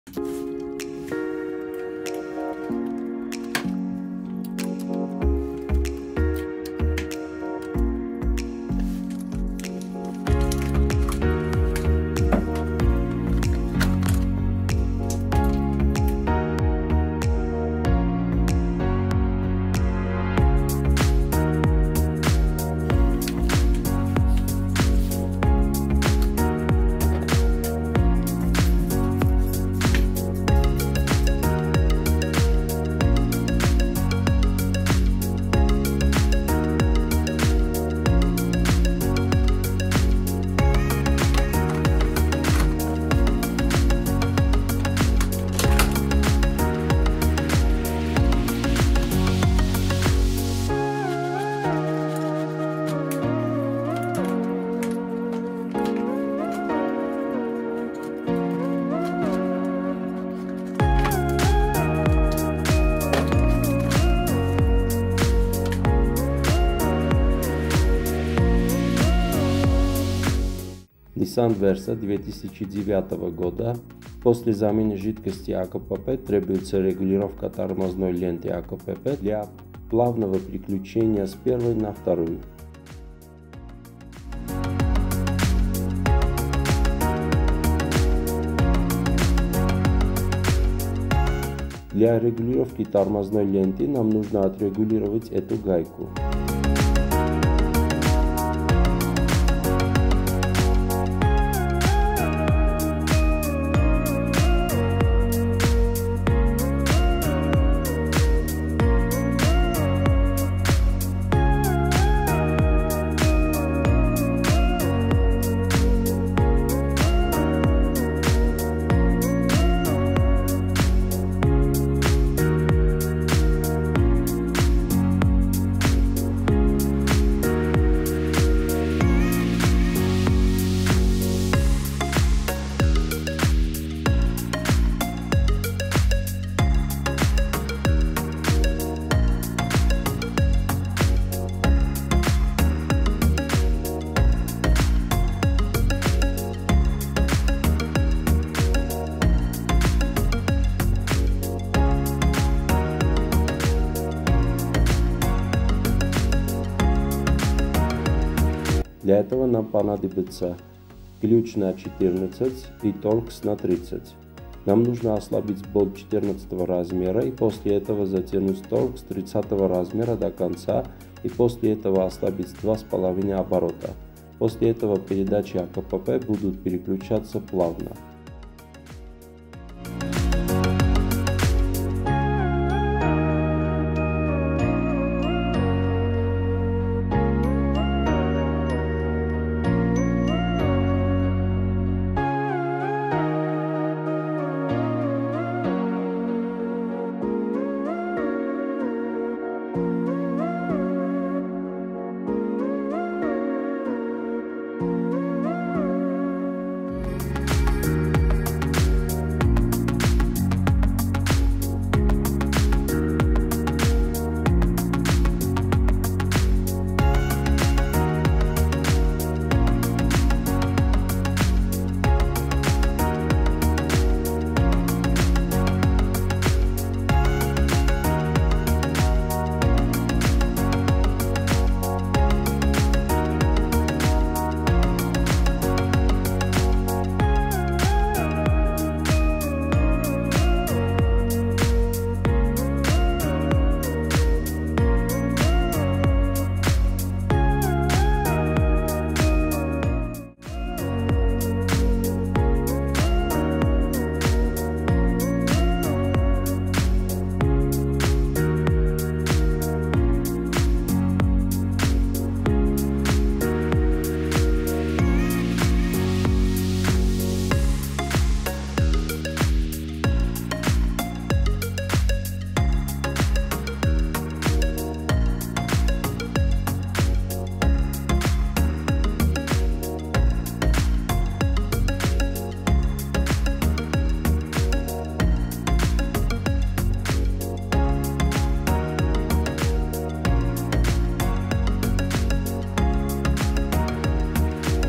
Kак убрать пинки акпп Nissan
Nissan versa 2009 года, после замены жидкости АКПП требуется регулировка тормозной ленты АКПП для плавного переключения с первой на вторую. Для регулировки тормозной ленты нам нужно отрегулировать эту гайку. Для этого нам понадобится ключ на 14 и торкс на 30. Нам нужно ослабить болт 14 размера и после этого затянуть торкс 30 размера до конца и после этого ослабить два с половиной оборота. После этого передачи AKPP будут переключаться плавно.